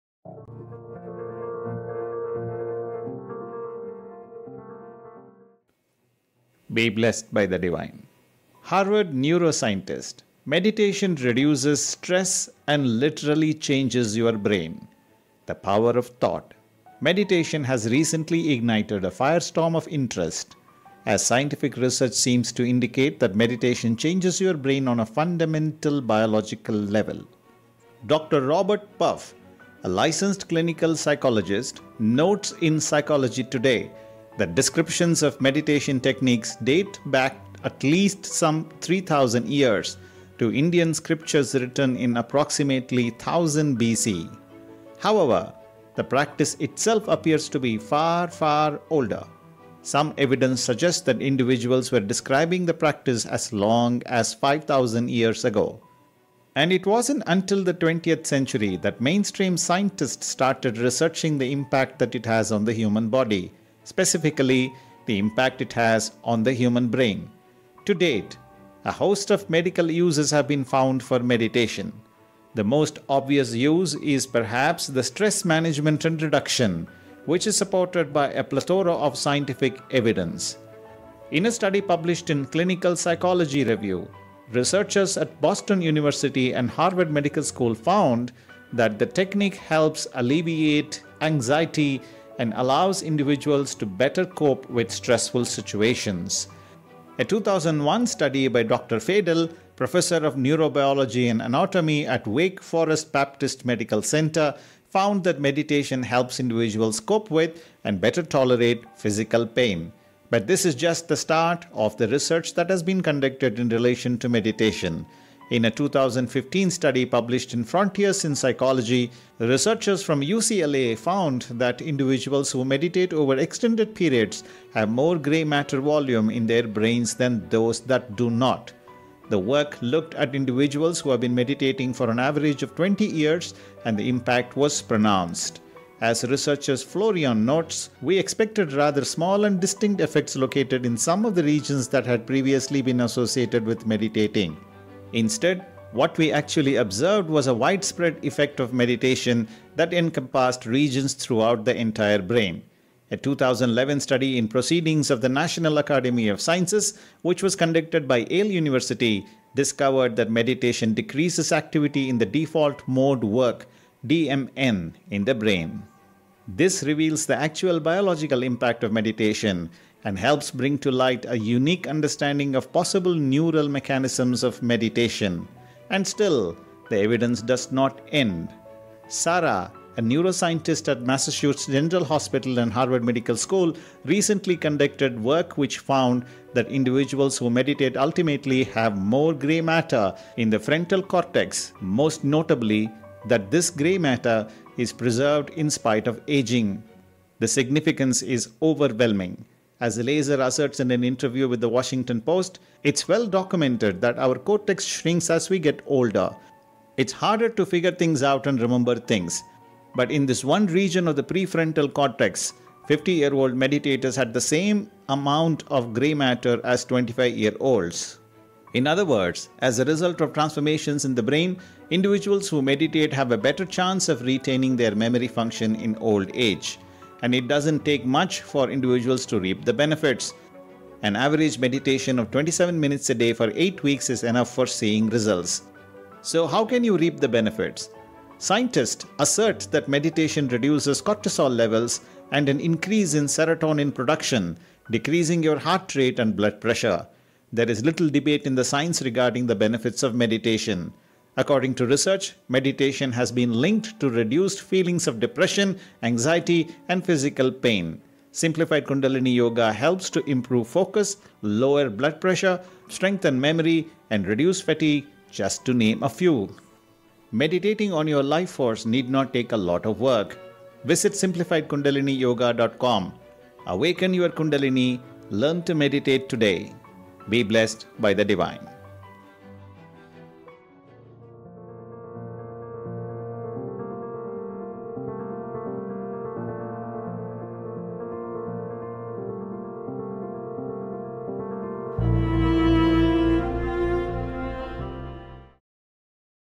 Harvard Neuroscientist: Meditation Reduces Stress and Literally Changes Your Brain
Harvard Neuroscientist:
Meditation Reduces Stress and
Literally Changes Your Brain

THE POWER OF THOUGHT

Meditation has recently ignited
a firestorm of interest,
as scientific research seems to
indicate that meditation changes
your brain on a fundamental,
biological level.

Dr. Robert Puff, a licensed
clinical psychologist, notes in
Psychology Today that descriptions of
meditation techniques date back
at least some 3,000 years,
to Indian scriptures written in
approximately 1000 BCE.

However, the practice itself
appears to be far, far older.

Some evidence suggests that individuals
were describing the practice
as long as 5,000 years ago.

And it wasn’t until the 20th century
that mainstream scientists started
researching the impact that
it has on the human body—
specifically, the impact it has
on the human brain.

To date, a host of medical uses
have been found for meditation.

The most obvious use is, perhaps,
stress management and reduction,
which is supported by a
plethora of scientific evidence.

In a study published in
Clinical Psychology Review,
researchers at Boston University and
Harvard Medical School found that

the technique helps alleviate anxiety
and allows individuals to better
cope with stressful situations.

A 2001 study by Dr. Fadel Zeidan,
professor of neurobiology and anatomy
at Wake Forest Baptist Medical Center,
found that meditation helps
individuals cope with, and better
tolerate, physical pain.

But this is just the start of
the research that has been
conducted in relation to meditation.

In a 2015 study published in
Frontiers in Psychology, researchers
from UCLA found that individuals
who meditate over extended periods
have more gray matter volume
in their brains than those that do not.

The work looked at individuals
who been meditating for an
average of 20 years, and the
impact was pronounced.

As researcher Florian Kurth notes:
We expected rather small and
distinct effects located in some
of the regions that had previously
been associated with meditating.

Instead, what we actually observed
was a widespread effect of meditation
that encompassed regions
throughout the entire brain.

A 2011 study in Proceedings of the
National Academy of Sciences, which
was conducted by Yale University,
discovered that meditation decreases
activity in the default mode network
(DMN) in the brain.

This reveals the actual biological
impact of meditation and helps
bring to light “a unique
understanding of possible
neural mechanisms of meditation.”

And still, the evidence does not end.

Sara Lazar, a neuroscientist at
Massachusetts General Hospital
and Harvard Medical School,
recently conducted work which found

that individuals who meditate
ultimately have more gray matter
in the frontal cortex and,
most notably, that this gray matter
is preserved in spite of aging.

The significance is overwhelming.

As Lazar asserts in an interview
with the Washington Post,
“It’s well-documented that our
cortex shrinks as we get older –

it’s harder to figure things out
and remember things.

But in this one region of the
prefrontal cortex, 50-year-old
meditators had the same amount
of gray matter as 25-year-olds.”

In other words, as a result of
transformations in the brain,
individual who meditate have a
better chance of retaining
their memory function in old age.

And it doesn’t take much for
individuals to reap the benefits.

An average meditation of
27 minutes a day for 8 weeks
is enough for seeing results.

So, how can you reap the benefits?

Scientists assert that meditation
reduces cortisol levels and
an increase in serotonin production,
decreasing your heart rate
and blood pressure.

There’s little debate in
the science regarding
the benefits of meditation.

According to research,
meditation has been linked
to reduced feelings of depression,
anxiety, and physical pain.

Simplified Kundalini Yoga helps to
improve focus, lower blood pressure,
strengthen memory, and reduce fatique,

Meditating on your life force
need not take a lot of work.

Visit SimplifiedKundaliniYoga.com
Learn to Meditate today.

Be Blessed by the Divine,
Krish Murali Eswar.